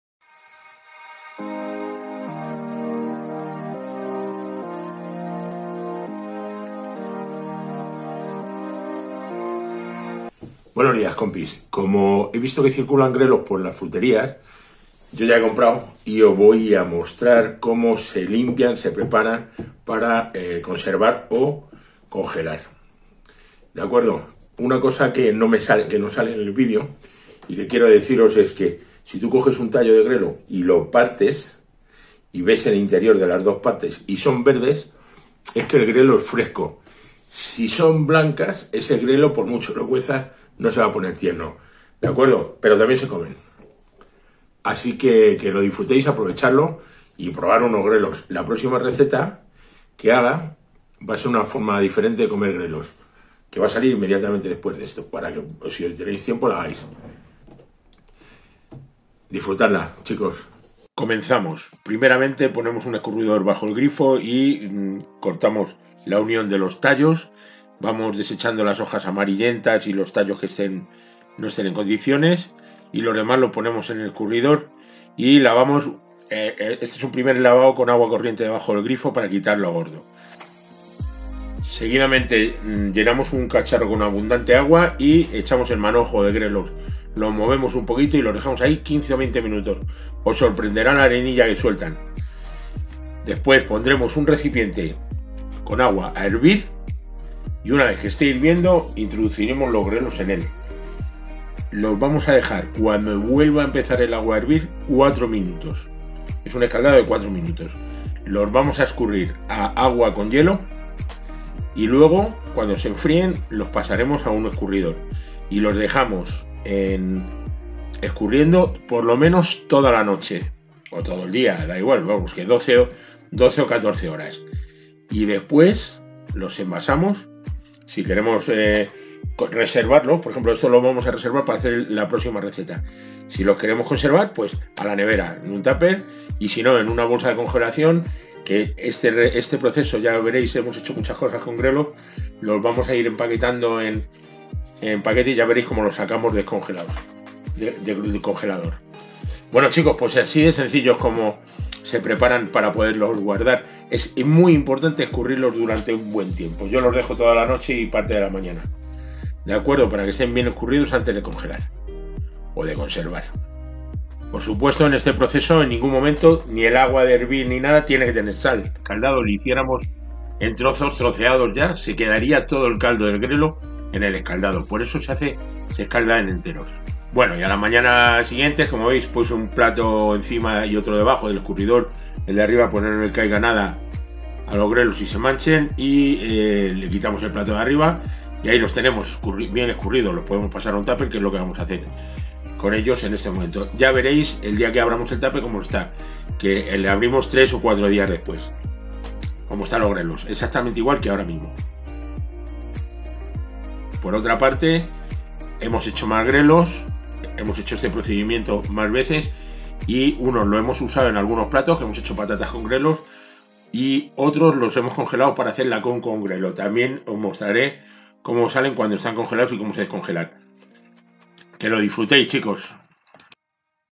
GRELOS COMO LIMPIAR Y PREPARAR PARA CONSERVAR O CONGELAR
Grelos, una verdura riquisima y con grandes propiedades para la salud,  hay que difrutar de ellos y aprobechar la temporada, aqui os muestro como